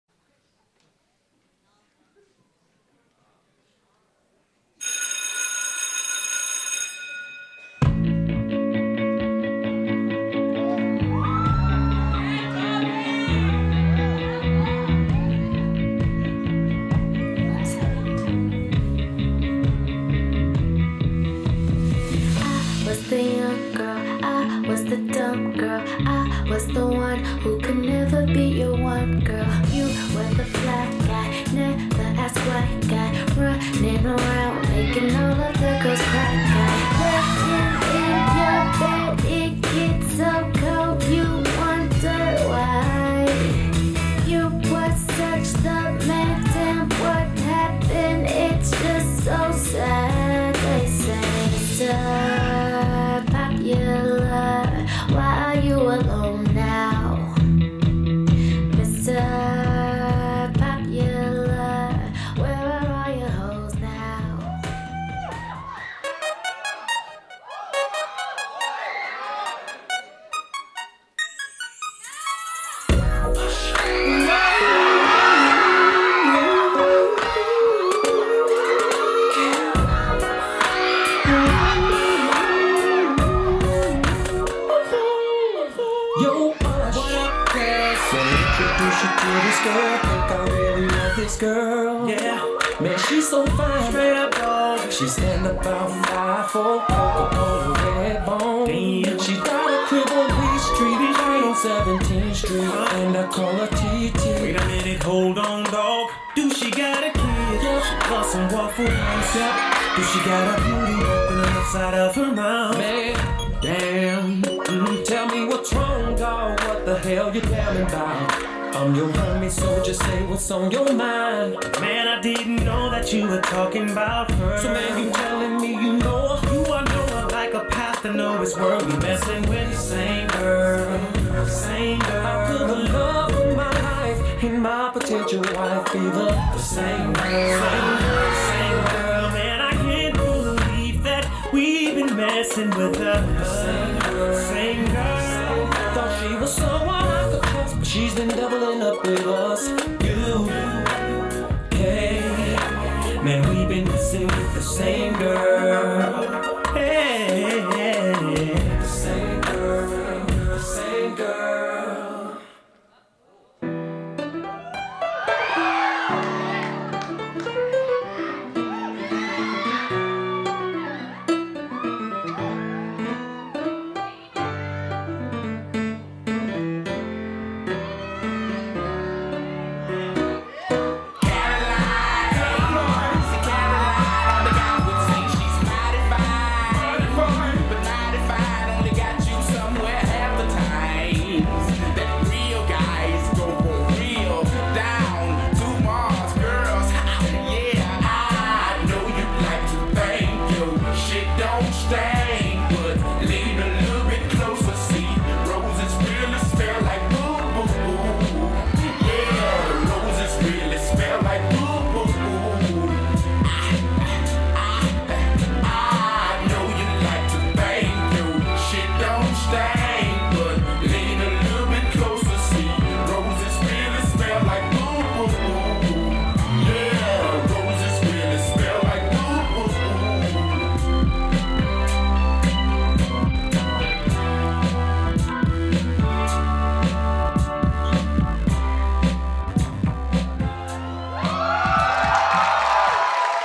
Urban Groove Presents Who Killed the Beat?: The Story Behind a Rose
She gets played, so she starts playing the game. But when you hurt a Rose, beware of her thorns...
Choreographer: Gabriela
Dancers: Brenden, Chris, Debbie, Gaby, Hansen, Jason, Jessica, Kevin, Megan, Riham, Sam, Saya, Tommy, Yan